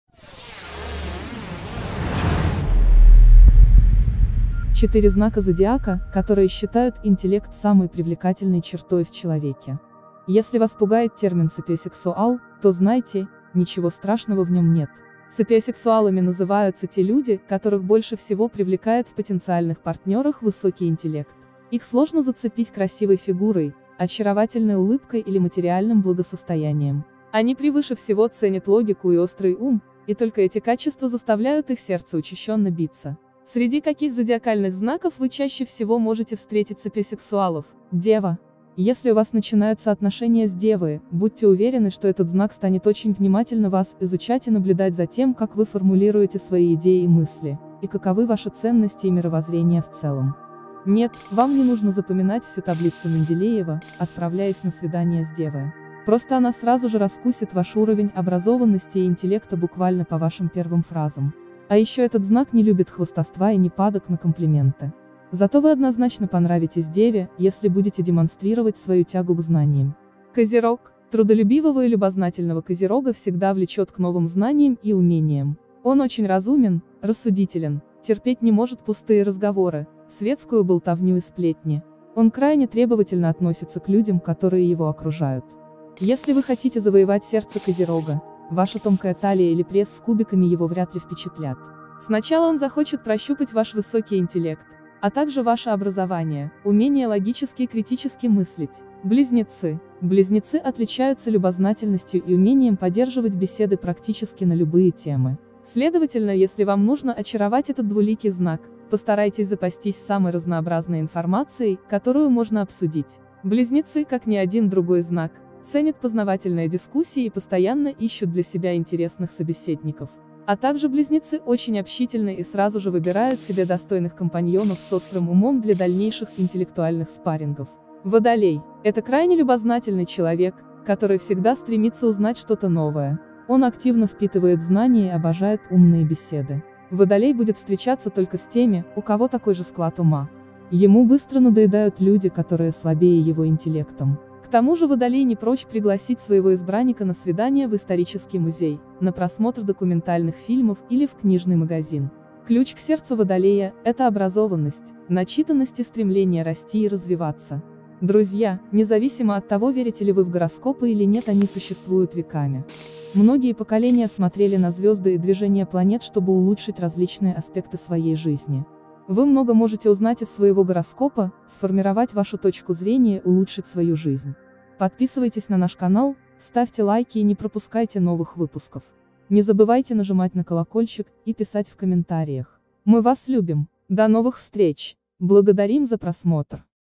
4 знака зодиака, которые считают интеллект самой привлекательной чертой в человеке
4 знака зодиака, которые считают интеллект самой привлекательной чертой в человеке

Если вас пугает термин «сапиосексуал», то знайте, ничего страшного в нём нет. Сапиосексуалами называются те люди, которых больше всего привлекает в потенциальных партнерах высокий интеллект. Их сложно «зацепить» красивой фигурой, очаровательной улыбкой или материальным благосостоянием. Они превыше всего ценят логику и острый ум, и только эти качества заставляют их сердца учащенно биться. Среди каких зодиакальных знаков вы чаще всего можете встретить сапиосексуалов?

00:00 Вступление
00:37Дева
01:10 Козерог
01:38 Близнецы
02:07 Водолей